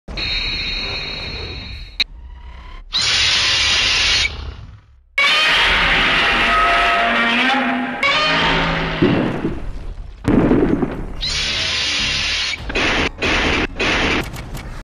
Godzilla and Mecha Godzilla roar at eachother but with showa sound effects
sounds all belong to Toho